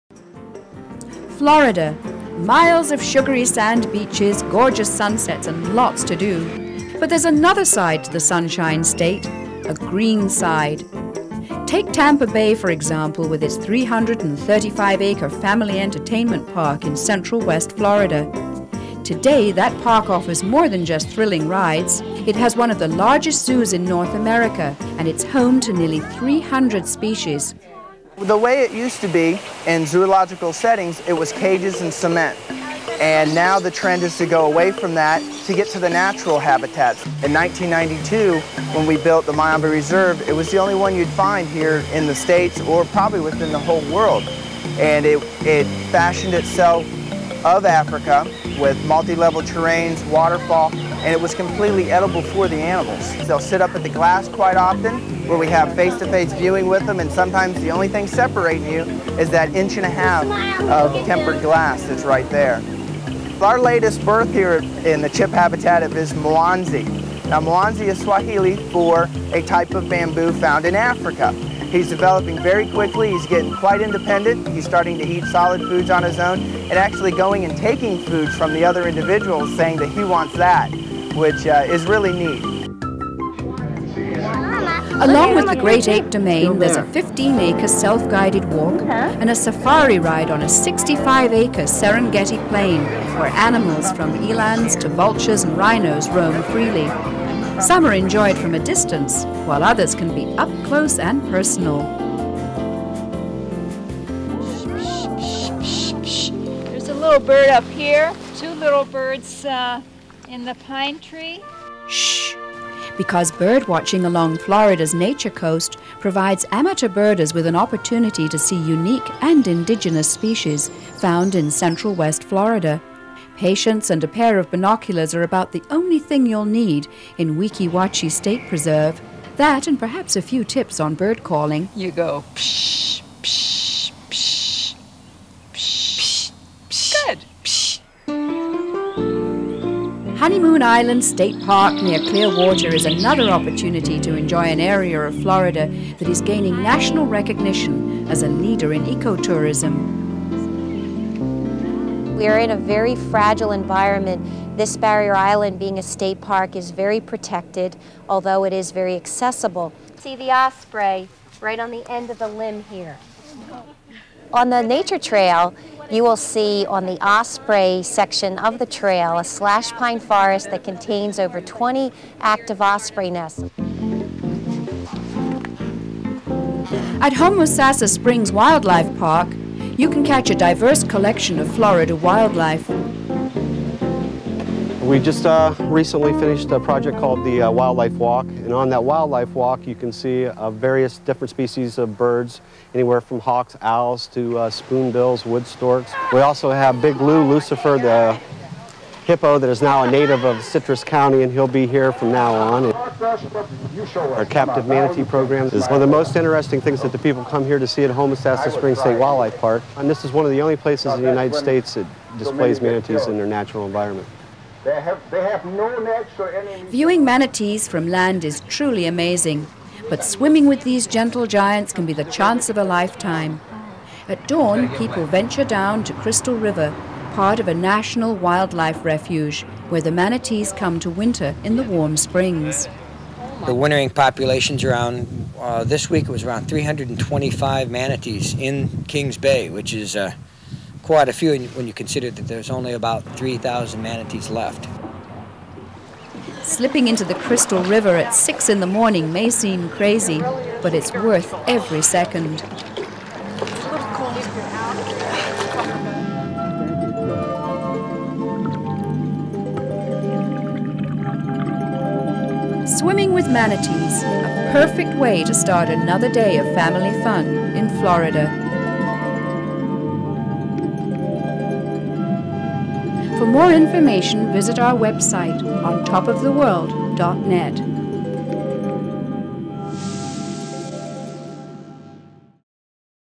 Florida -- Wet and Wild
Our search for wildlife takes us to Florida to visit one of the largest zoos in North America.  Here at Tampa Bay they built the Myombe Reserve, a natural habitat, providing a home for gorillas and chimpanzees. Travelling along the nature coast a member of the Audubon Society gives us lessons in bird calling and then it's off to the fragile environment of a barrier island near Clearwater for osprey-spotting.  At Homosassa Springs there are more birds and wildlife and a visit with a famous movie star, Big Lu, before we take an early morning swim with the manatees. and check out episode #66 in the International Category.